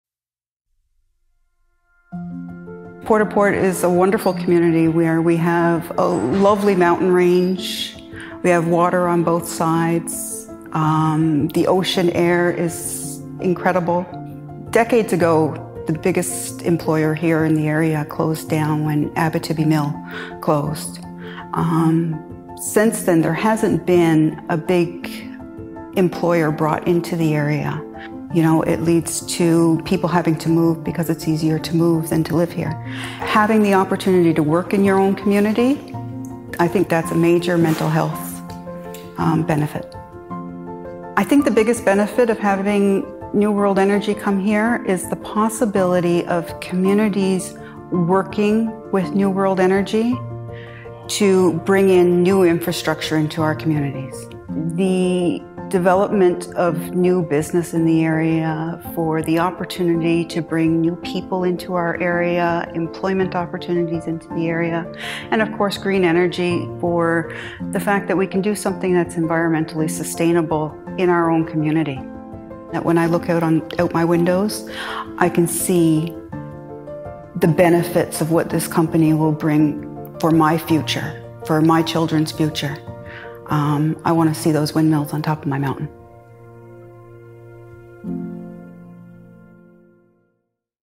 West Coast Proud: Jan Crane - Port au Port East, NL
Meet Jan Crane from Port au Port East, NL.

Local residents are optimistic about how the project – and the industry – can benefit their communities.

Video credit: Up Sky Down Films